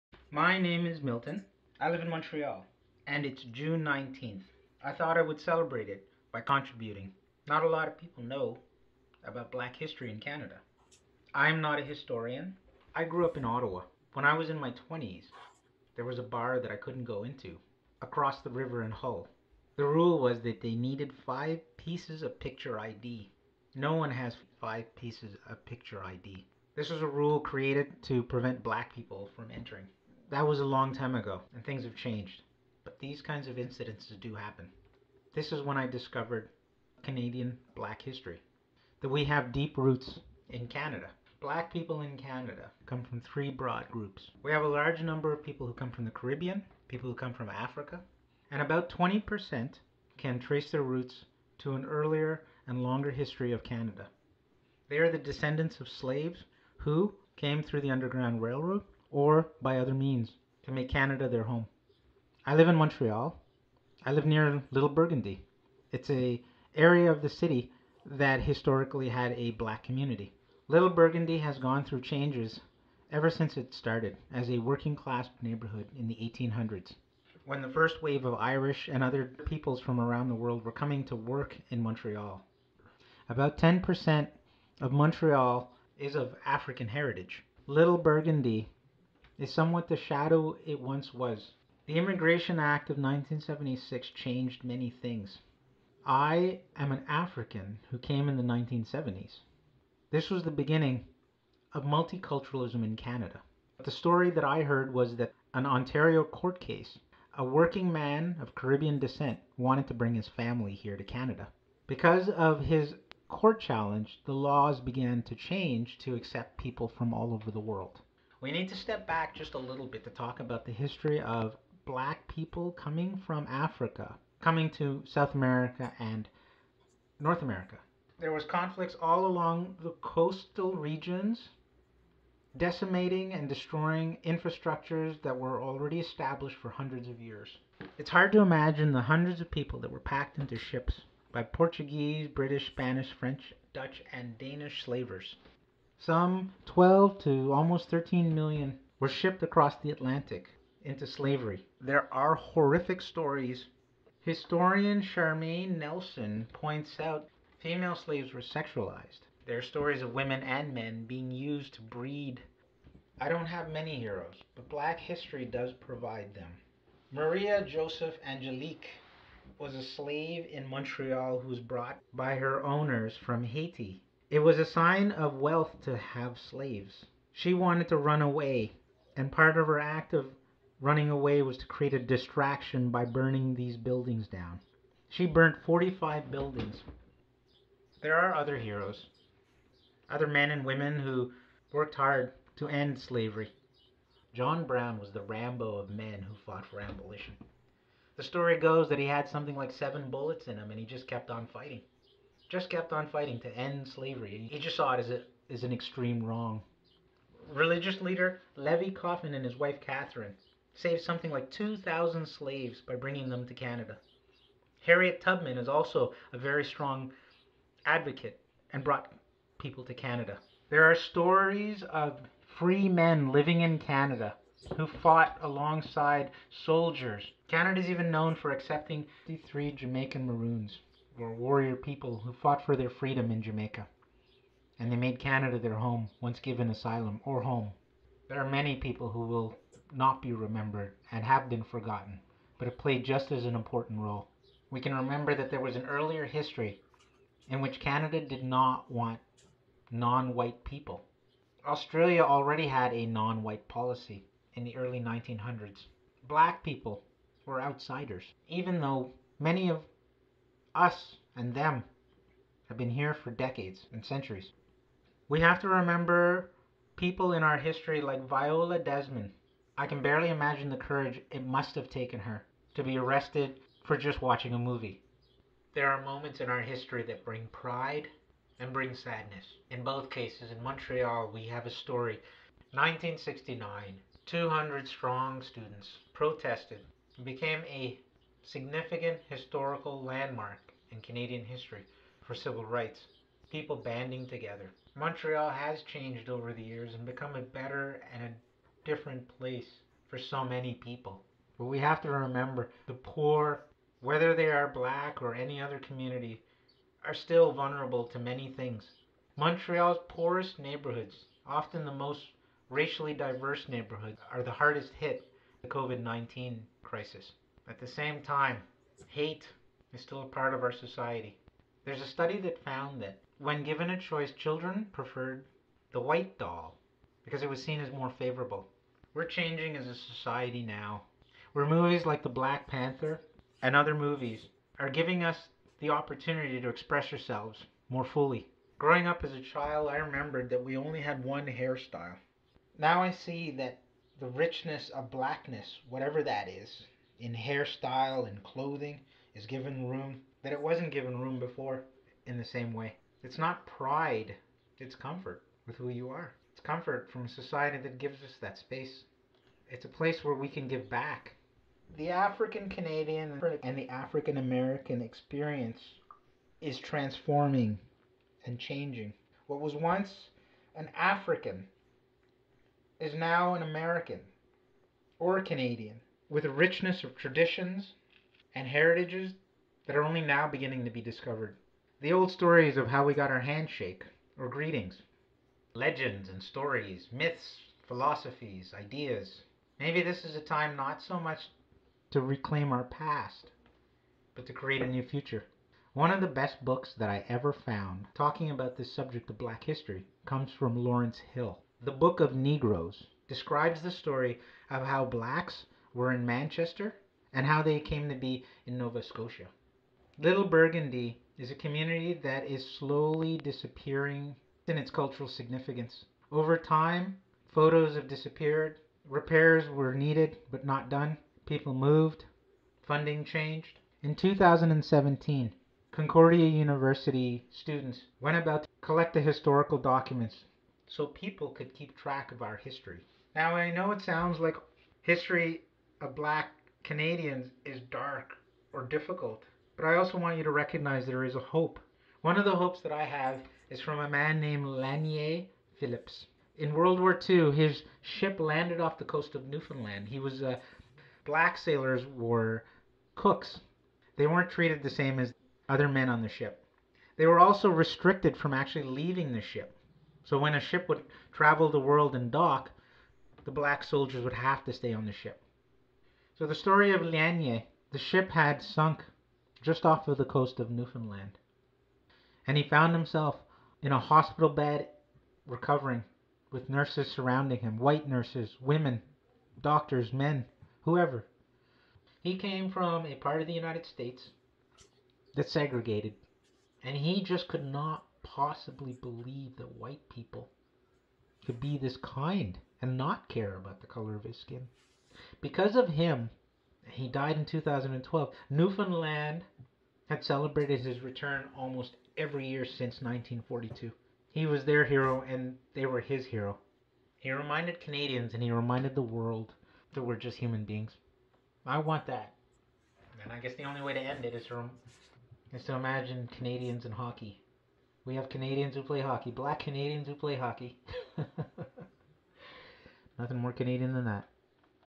Black History Canada
Seeing that this was June 19, I decided to create a video on the history of Canada. Usually, I focus on cannabis -- this is an exception. I recognize that in addition to the next few videos on medical cannabis, that perhaps I have other things to offer for now. I'm actually thinking of my nephew who has to grow up in this world he inherited. I think about the next generation. Here is a short history of how African people came to Canada.